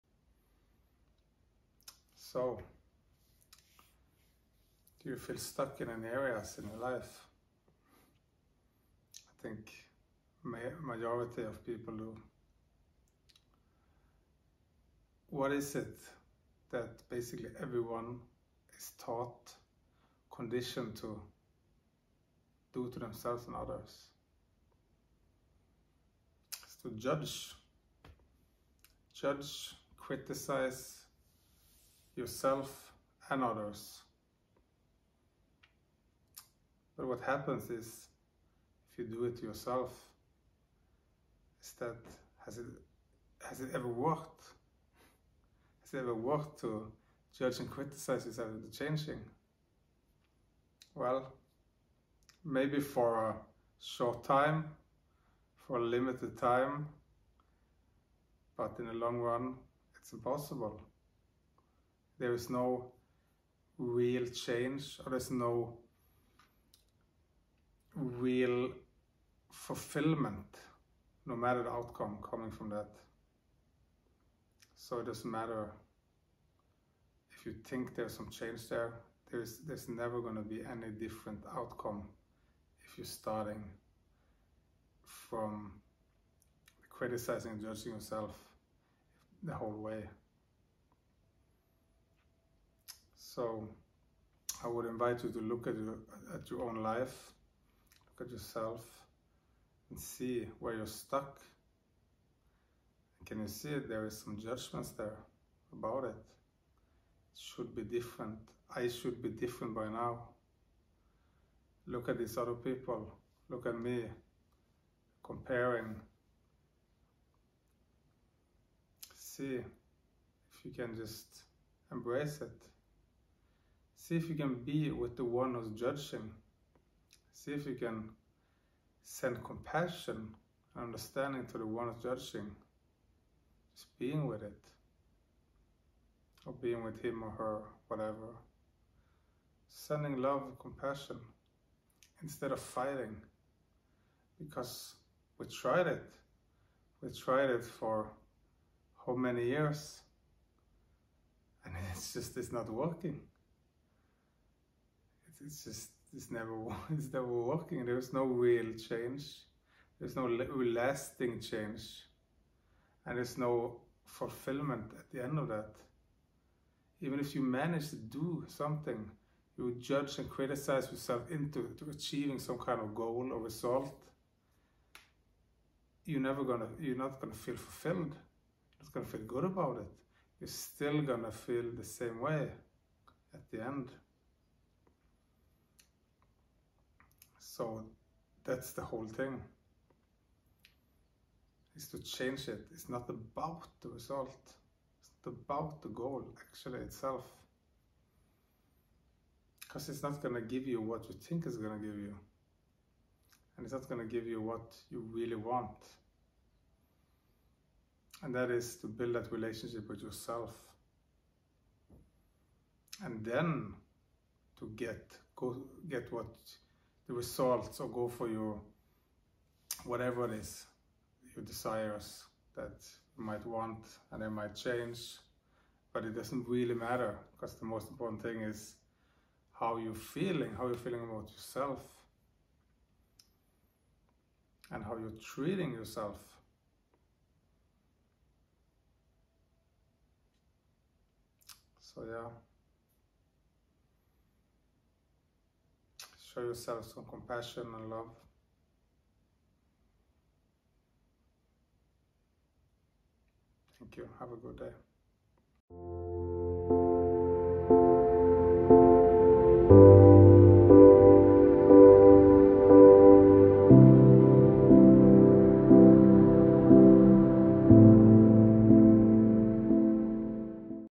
The 1 thing I found out that got me unstuck in life after many years!
I wanted to share with you what could be the most likely reason why you feel like you are stuck. This is one of the biggest reasons that keep people stuck in their life.

Why you feel stuck in life, is almost always related to this.
We judge ourself the harshest and can be the meanest person to ourselfs, that is quite the opposite of being your own biggest cheerleeder and supporter.
And you would just keep gaining the same kind of experiences you resist so much.